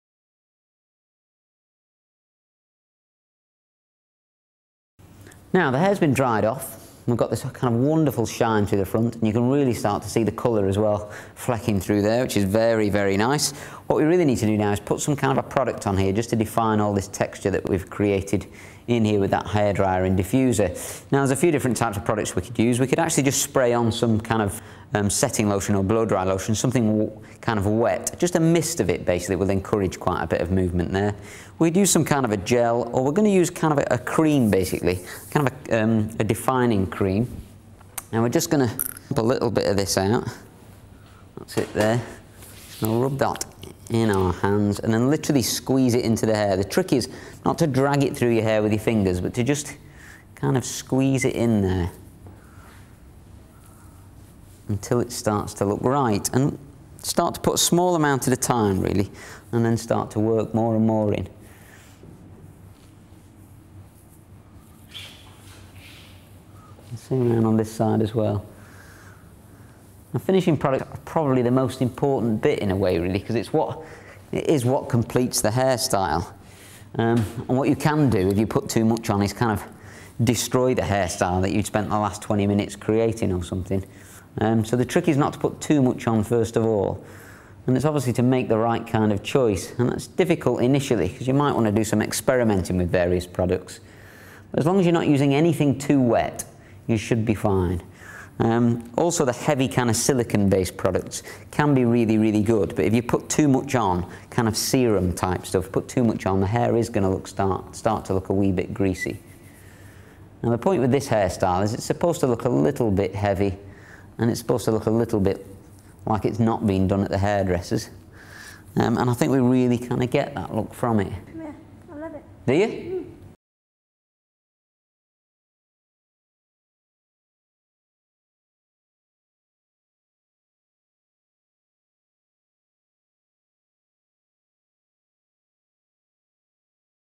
How To Style Short Dark Hair - Part 7 - Finishing I
Michael shows off his first style, working a cream into Charlottes hair